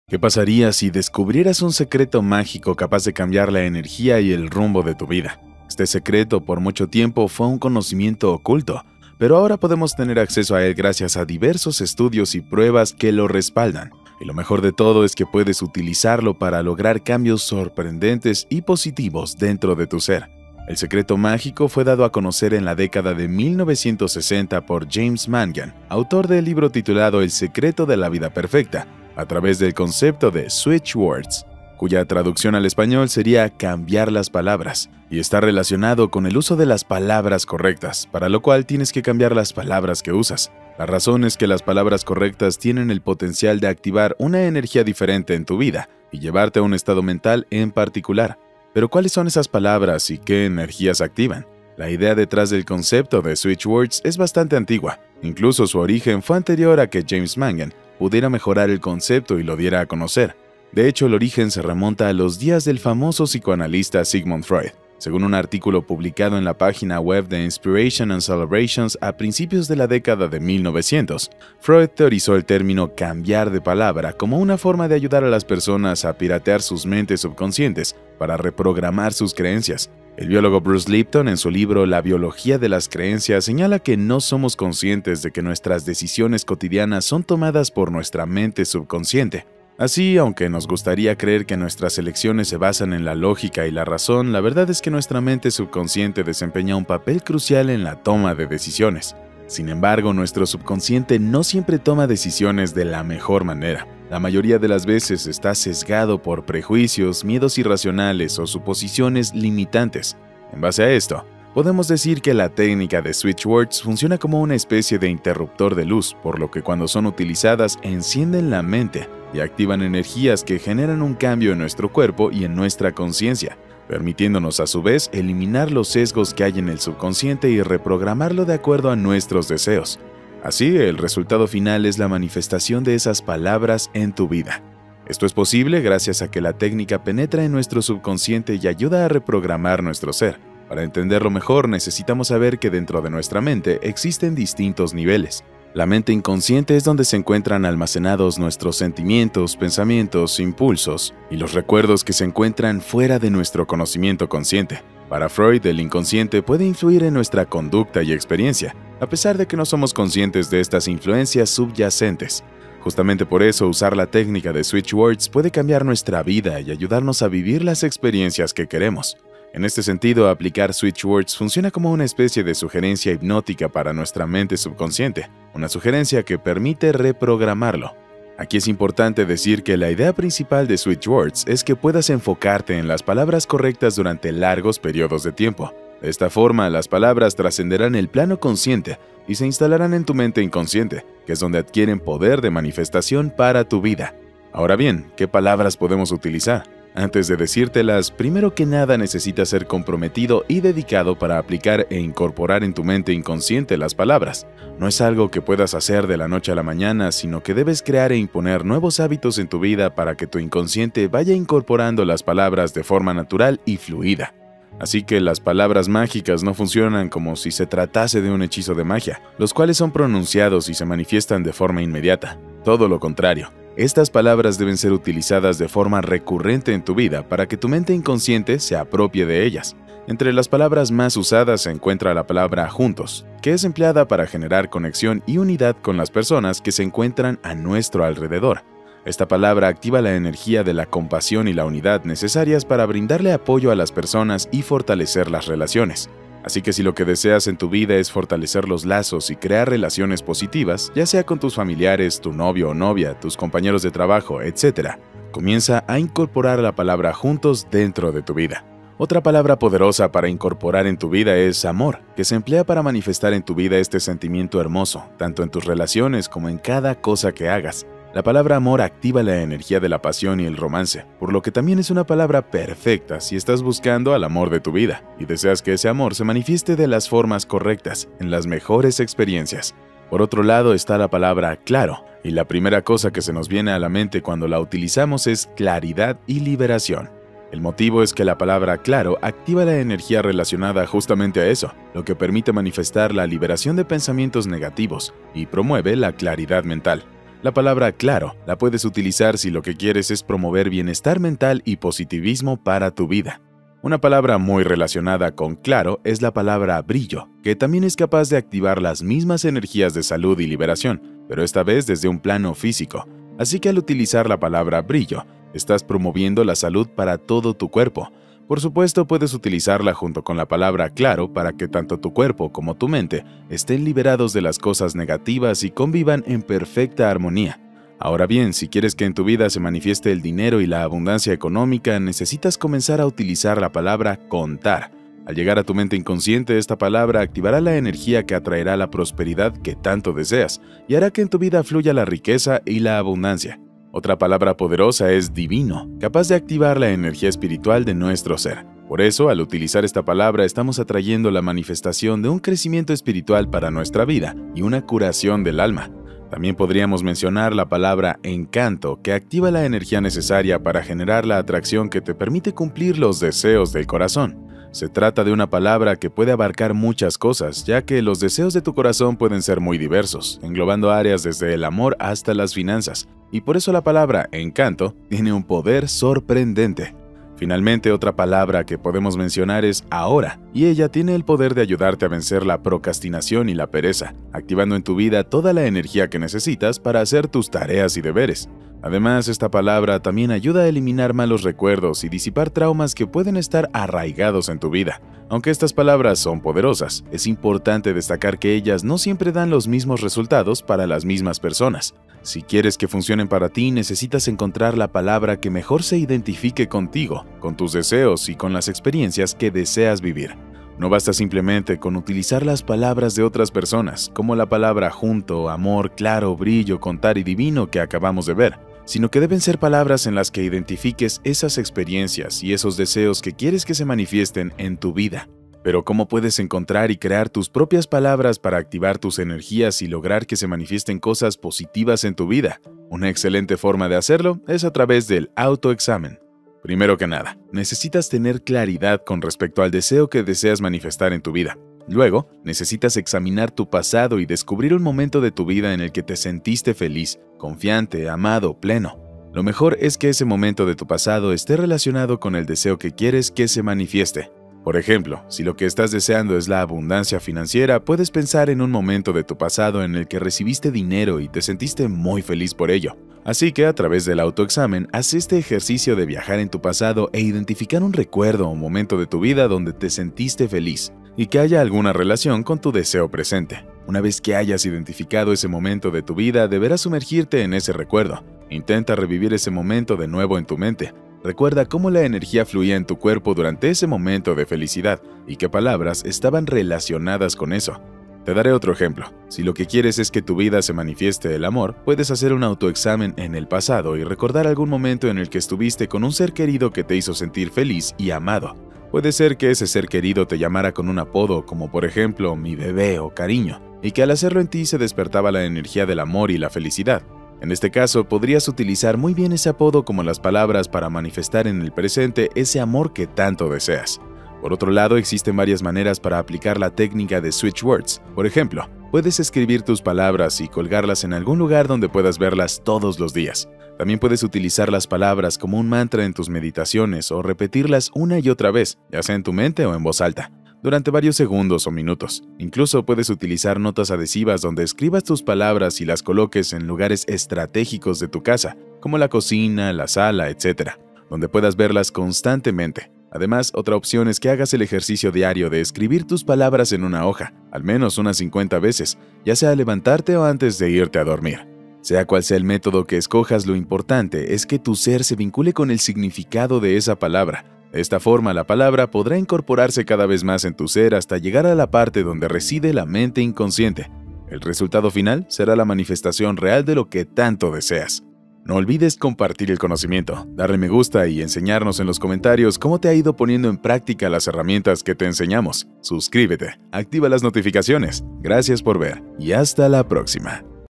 “Cada Palabra Activa Una Energía Diferente” - Conocimiento Oculto de Manifestación - (LoA)
🥇 ¿Qué pasaría si descubrieras un secreto mágico capaz de cambiar la energía y el rumbo de tu vida? Este secreto por mucho tiempo fue un conocimiento oculto, pero ahora podemos tener acceso a él gracias a diversos estudios y pruebas que lo respaldan.  Y lo mejor de todo es que puedes utilizarlo, para lograr cambios sorprendentes y positivos dentro de tu ser.

¿Pero cuáles son esas palabras y qué energías activan?

¡Por favor comparte el CONOCIMIENTO!